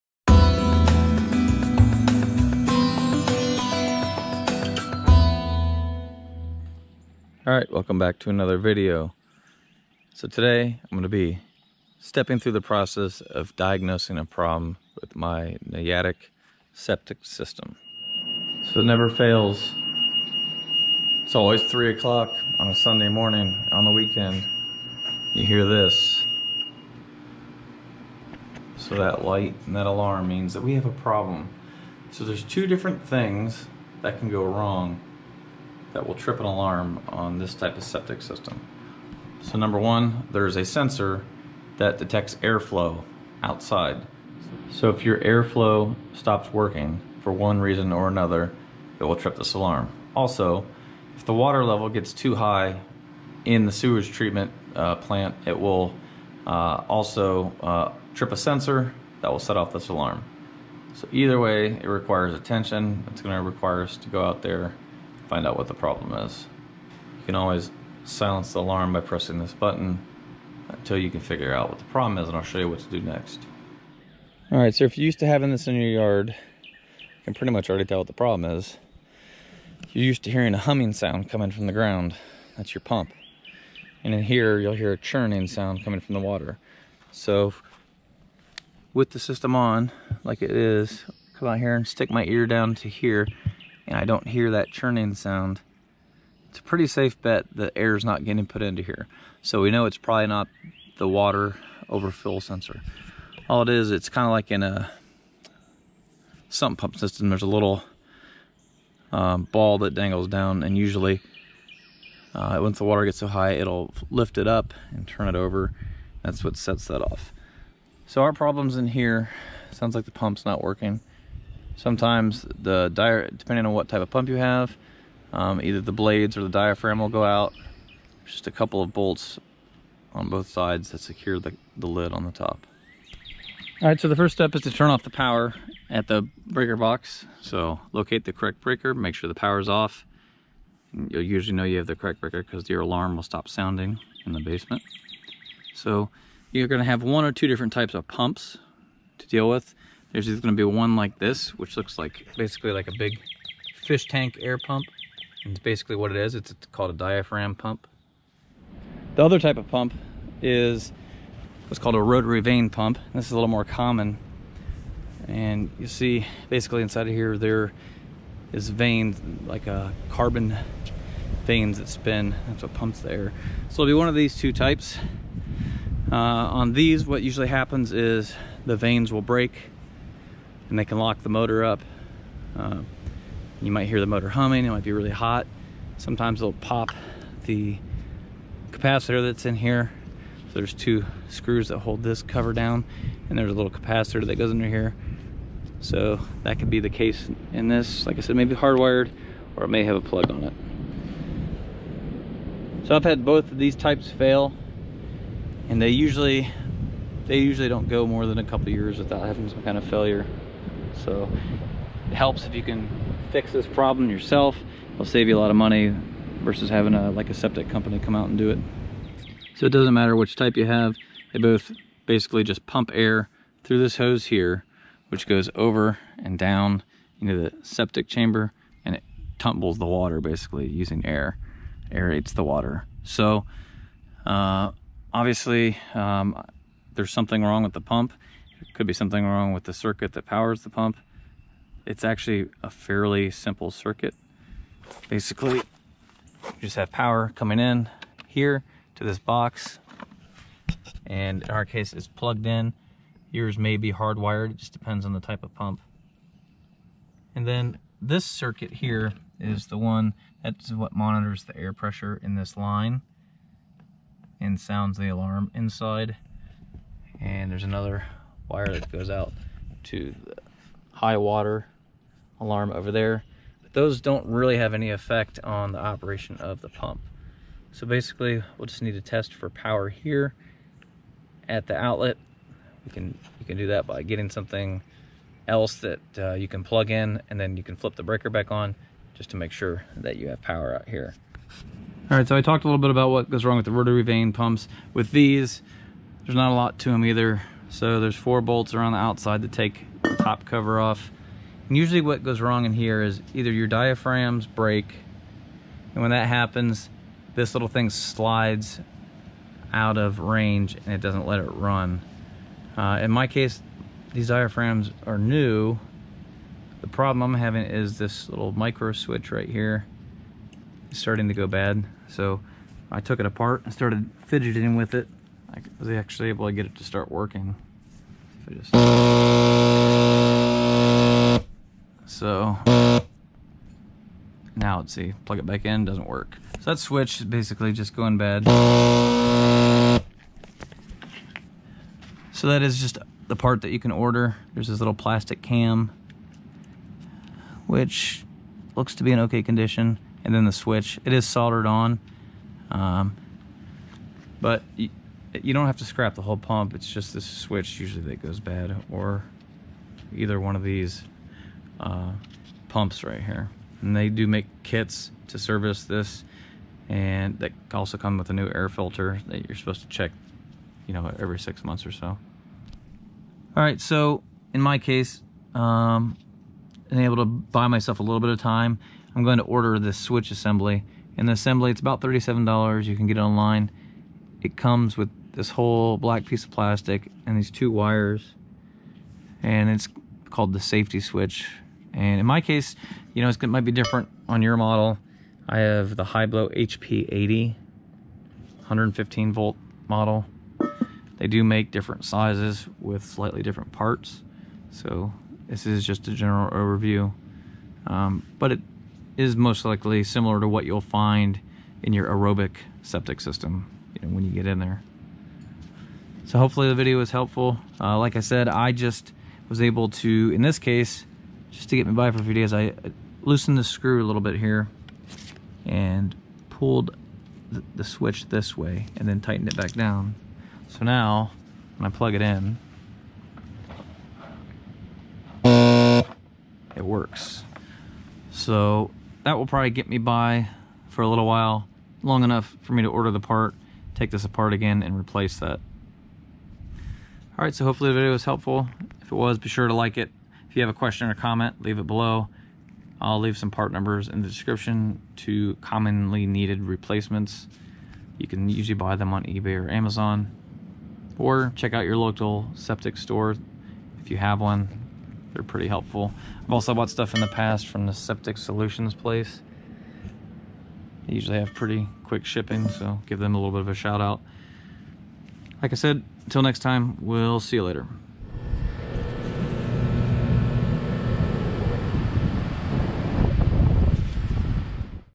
Aerobic Septic System Alarm Troubleshooting - Gast or Hiblow Pump Not Working
Today I'm going to show you what to do when your Nayadic or other aerobic septic system alarm goes off. These systems may seem complex, but they really are not.  You can take a few simple DIY steps to diagnose and repair the most common causes of septic airflow alarms. This applies to many brands of aerobic septic systems.  If you have a Hiblow linear or Gast rotary vane pump, I'll list some common replacement parts below. (Paid Amazon Links)
Gast Rotary Vane AT05 Pump
Hi-Blow HP-80 Linear Pump
Hiblow Repair Kit
Hiblow Modern Style Safety Switch Only
Gast Rotary Vane Repair Kit